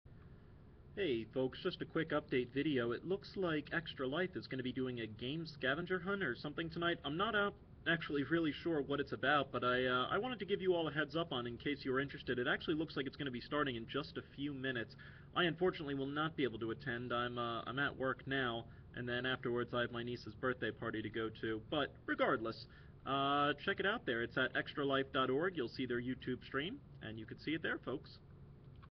Extra-Life.org Scavanger Hunt for Games...?
Honestly not really sure what it's about, but it's worth checking out I think.  I unfortunately won't be able to as I'm working now and have my nieces birthday party to go to after - but hey, might get yourself some free games.  Watch stream at: